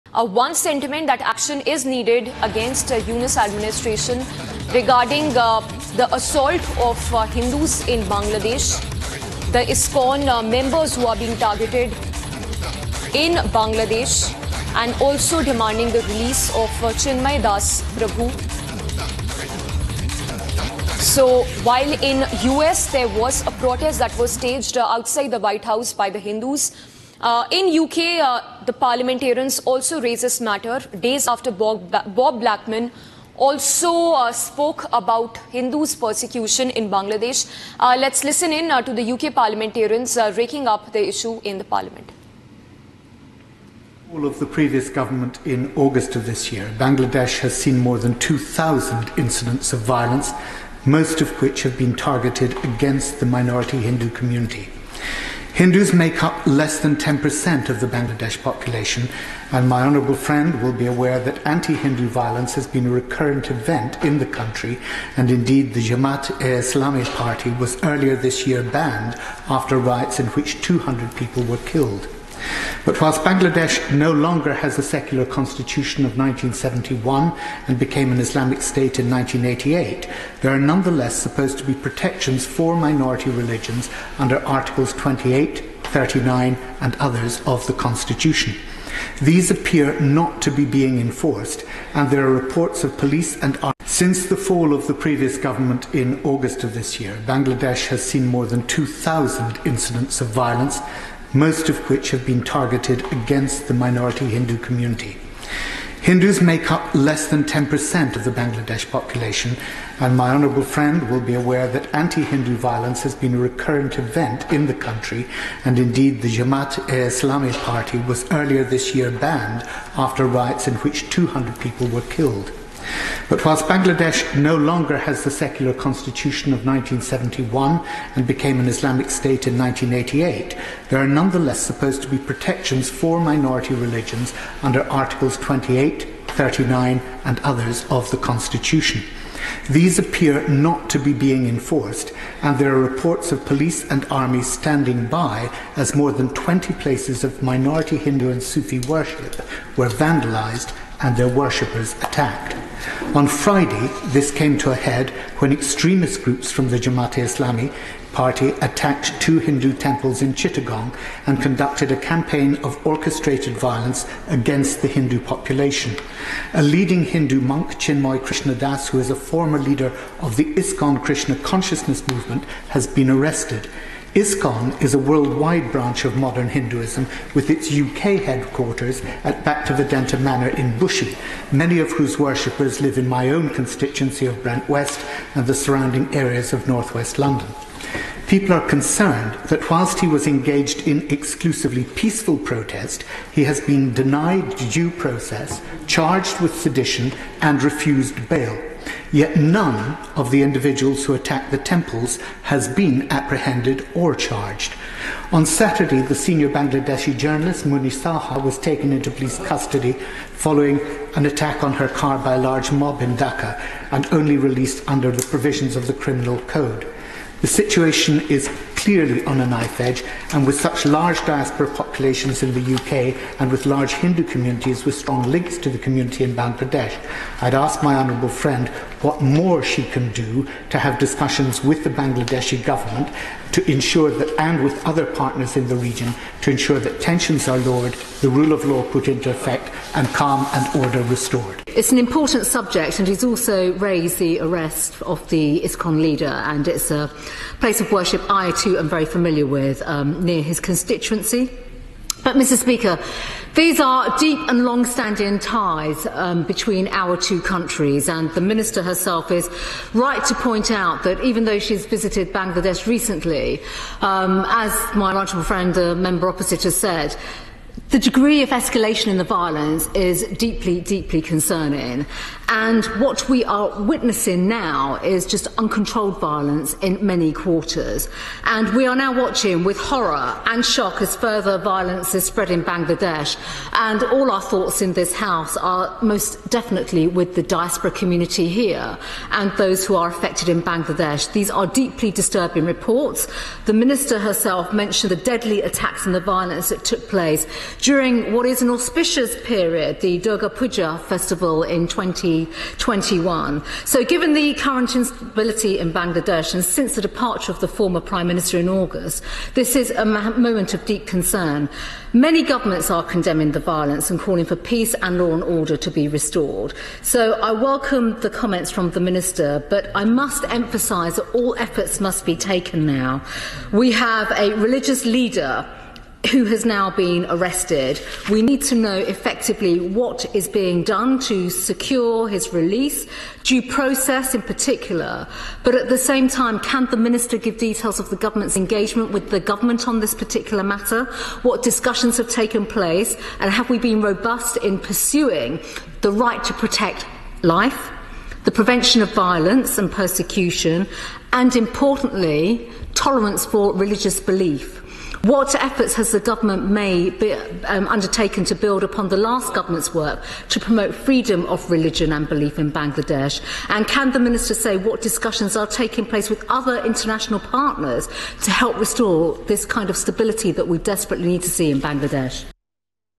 UK MPs Priti Patel, Barry Gardiner Condemn Attacks on Hindus Says, 'Situation Clearly on Knife Edge'
MP Barry Gardiner rakes up the atrocities against Hindu minority in Bangladesh. Member of Parliament Priti Patel too raised concerns over violence in Bangladesh and demanded Robust action needs to be taken to protect life and prevent violence and persecution..